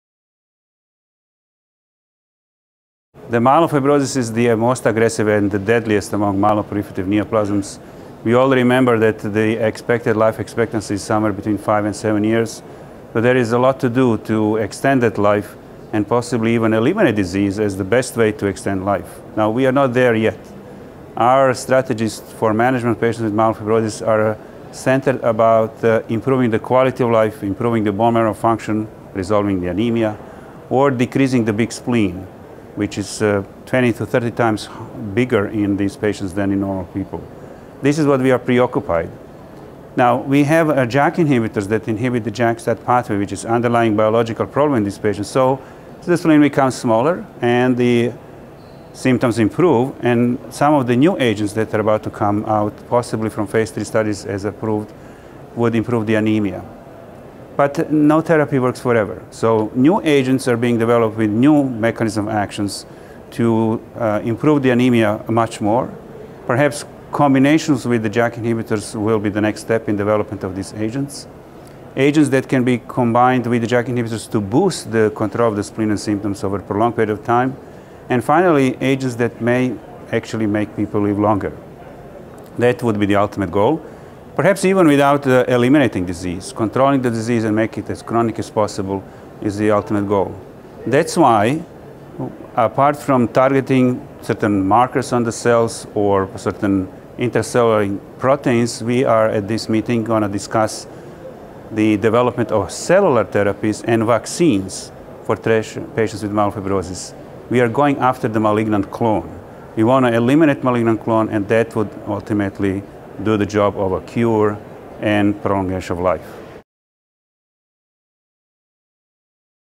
Novel treatment strategies in myelofibrosis
Srdan Verstovsek, MD, PhD, The University of Texas MD Anderson Cancer Center, Houston, TX, gives an overview of treatment strategies for patients with myelofibrosis (MF), highlighting the importance of novel JAK inhibitors and the promise of combination approaches. Dr Verstovsek also comments on the potential application of cellular therapies and vaccines in the future. This interview took place at the Tenth Annual Meeting of the Society of Hematologic Oncology (SOHO) held in Houston, TX.